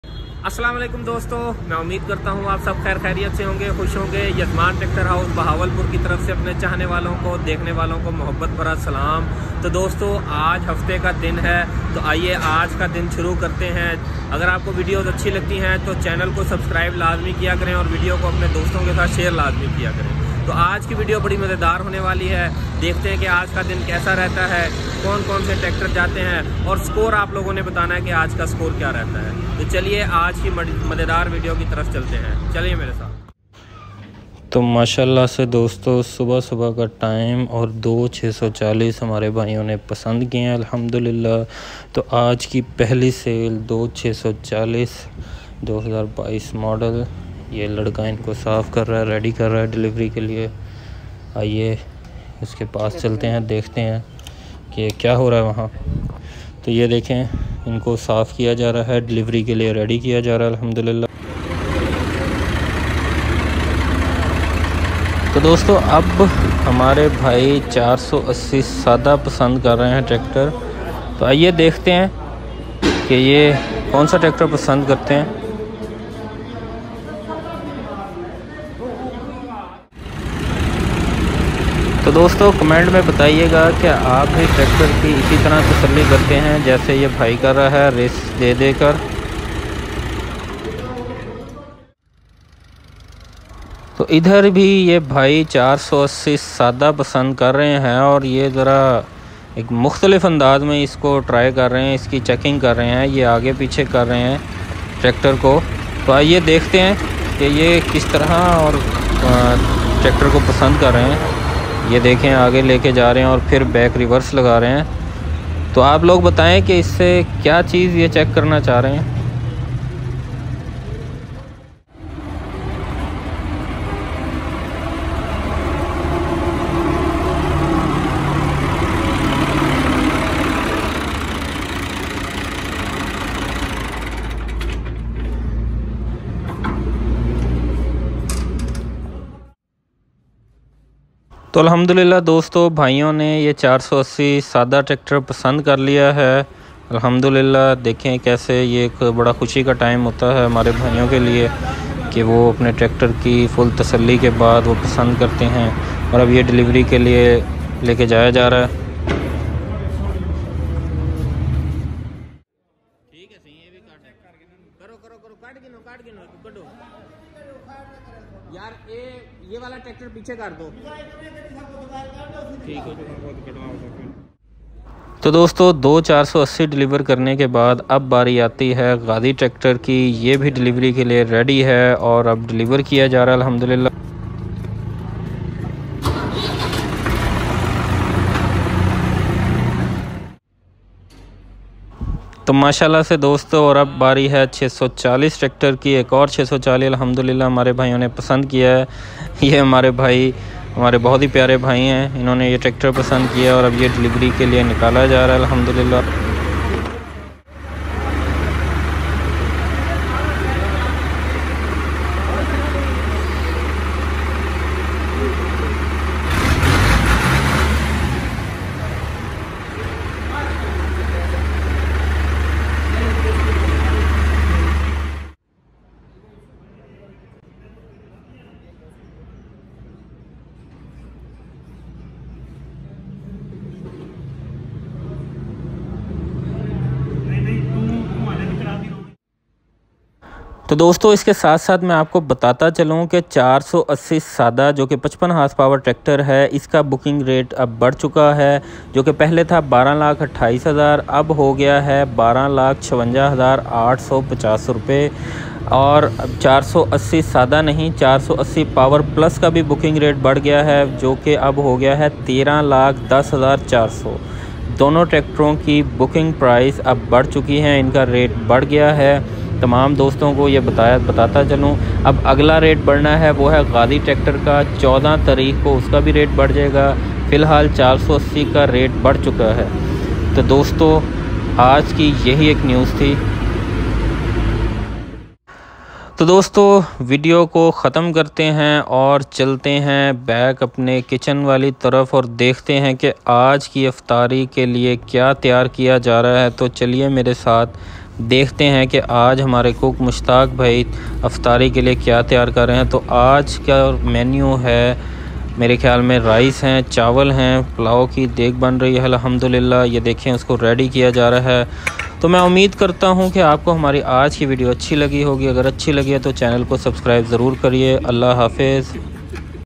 fiat 480 new prices and today sale | tractor video | yazman tractorhouse
New price of nh 480/s and 480 power plus and today sale